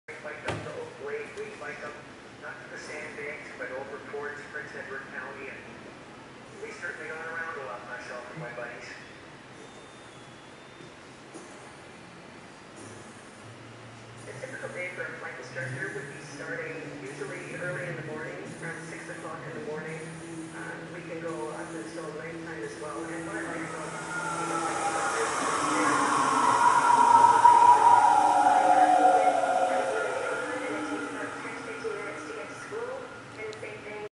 Handcar Interactive - RMA Technologies Inc.
Oscillating handcar handle
Video playback speed set to synchronize with handle pumping
Motor driver wheels rotate while pumping
Digital encoder to measure speed
ScanAndPlay™ media player system